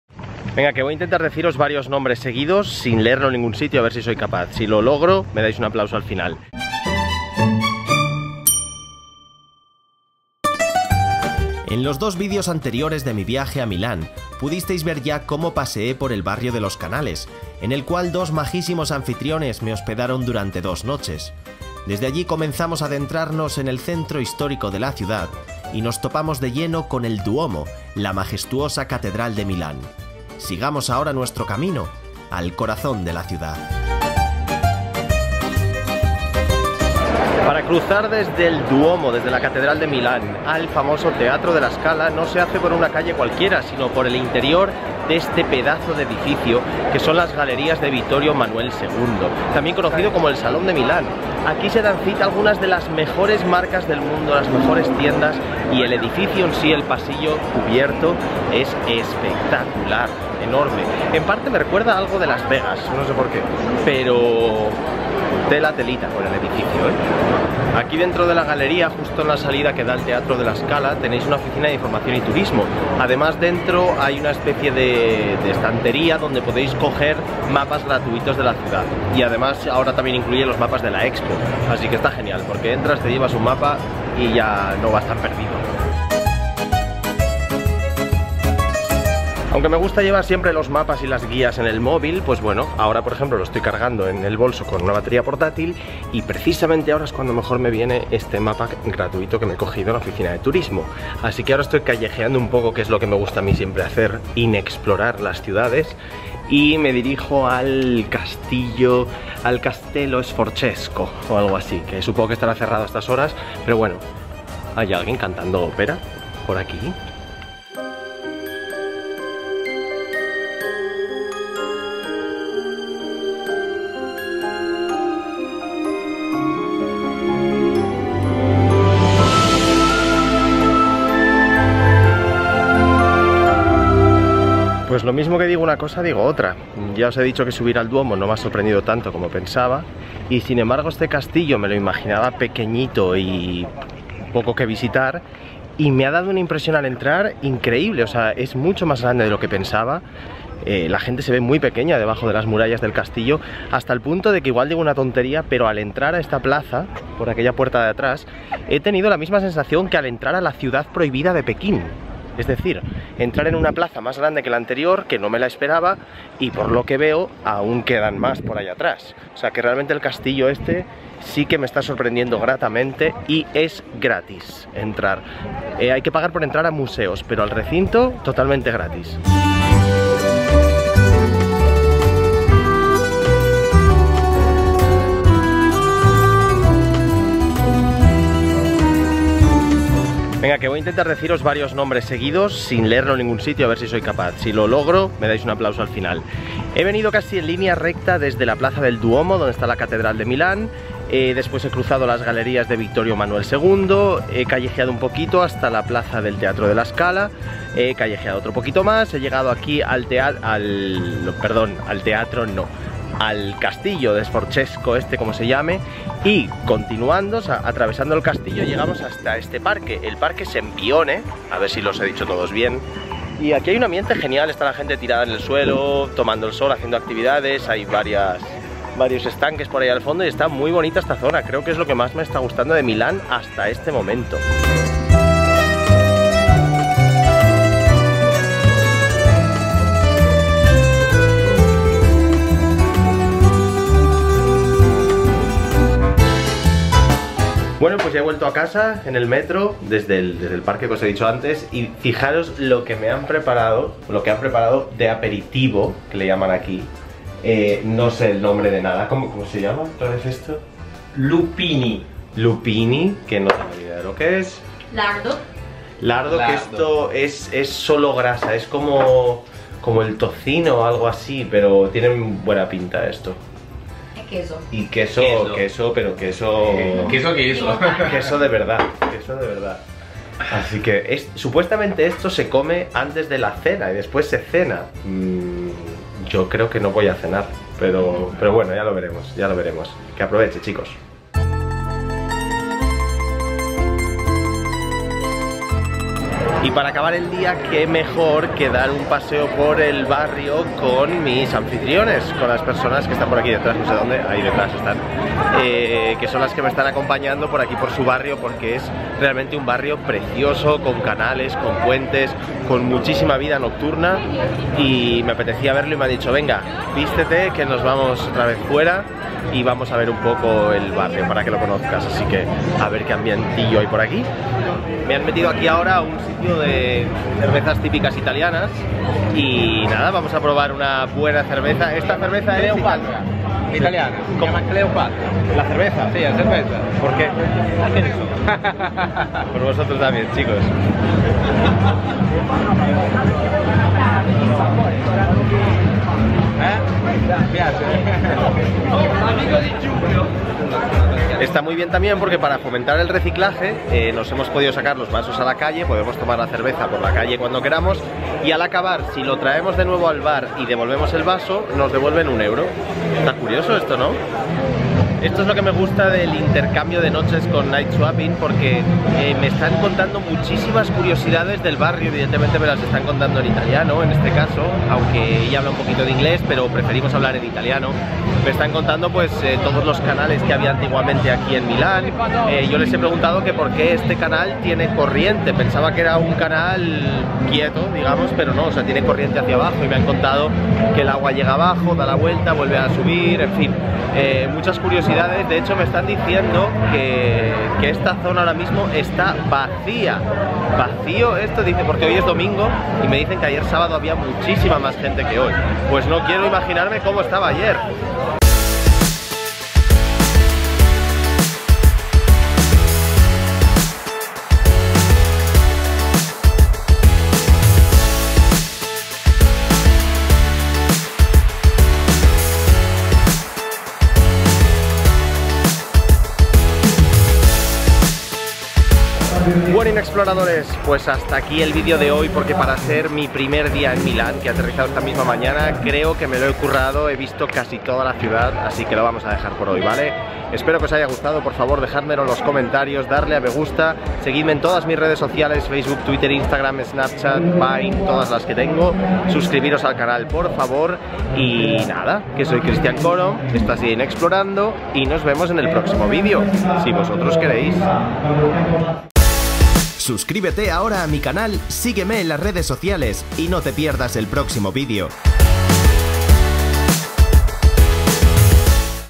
El gran castillo Sforzesco | MILÁN, ITALIA 3/3
Después de visitar El Duomo y pasear por el centro histórico de Milán en los videos anteriores, en este terminamos de visitar la ciudad paseando por la galería Vittorio Emanuele II hasta llegar al imponente Castello Sforzesco y disfrutar de la tranquilidad y los paisajes del parque Sempione. Quizás fue la parte que más me sorprendió de Milán.

Y para acabar el día, mis anfitriones de NightSwapping, la web de trueque de noches entre particulares, me llevaron a conocer la vida nocturna de su espectacular barrio y además me ofrecieron un genial aperitivo en su casa.